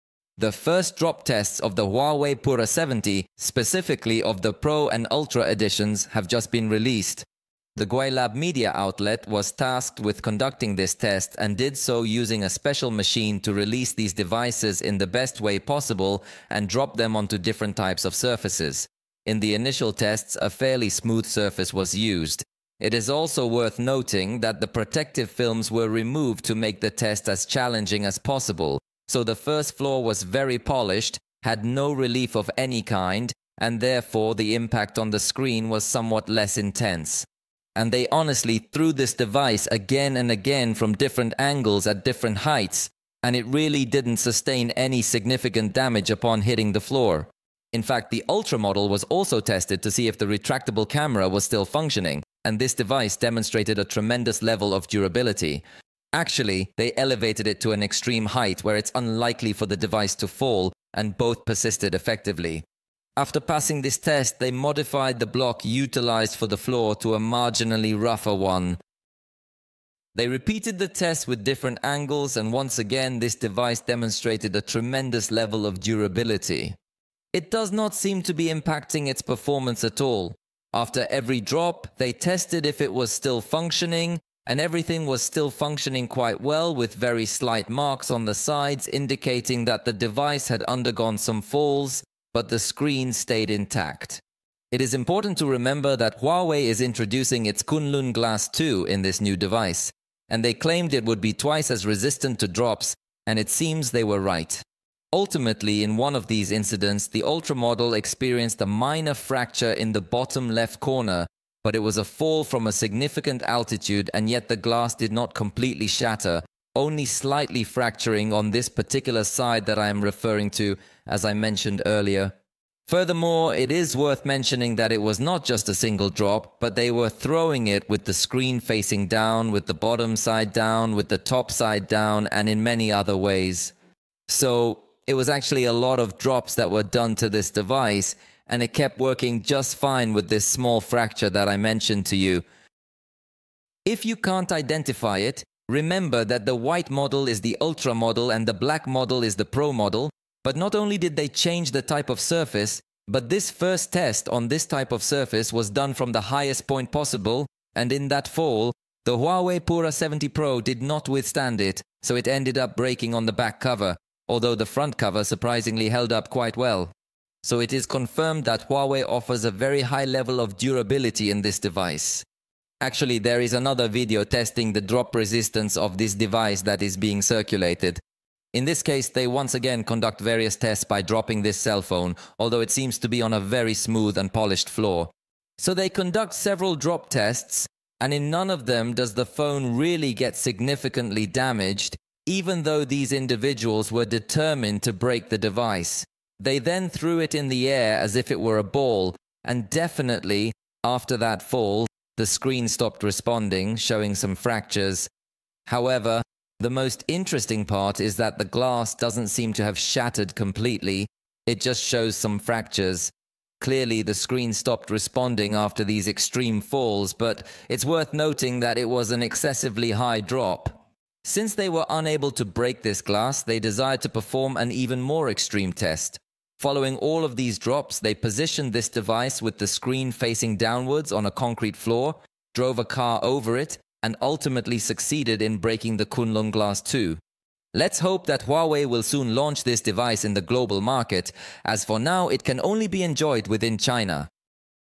HUAWEI Pura 70 Ultra FIRST DROP TEST is published
The time has come to see how resistant the new HUAWEI Pura 70 Ultra is through these tests